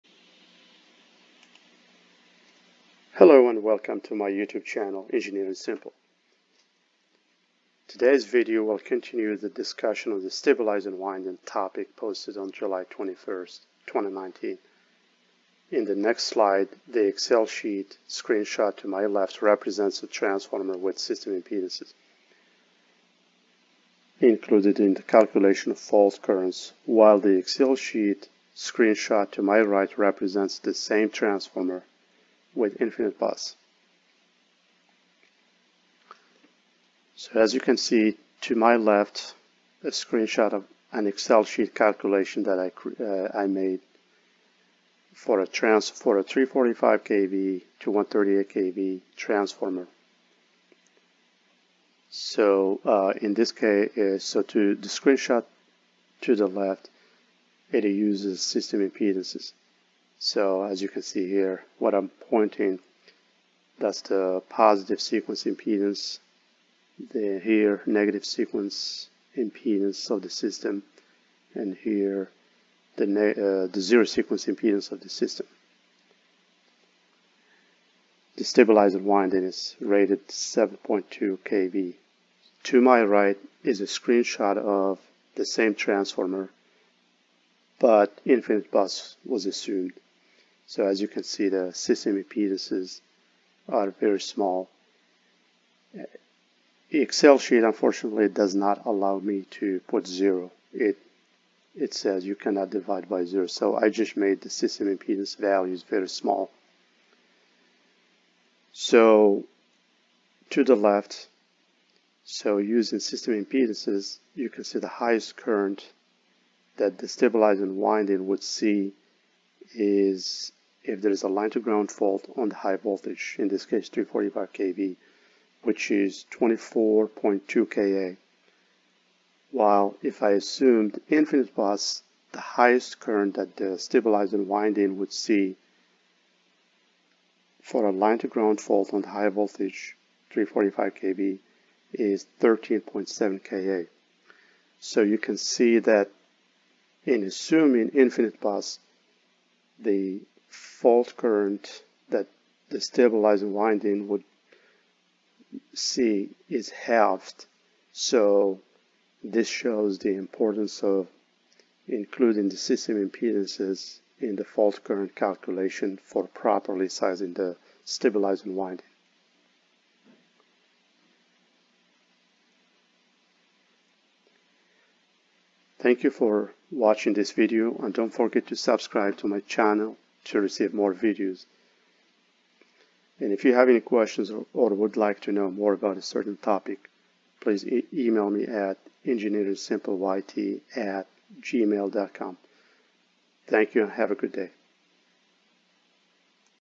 Transformer Stabilizing Winding (Buried Delta) - 2
Transformer stabilizing winding - buried delta.

Comparison of fault current calculation of a transformer stabilizing winding with system impedance included vs infinite bus. Note: this case is in respect to a transmission transformer.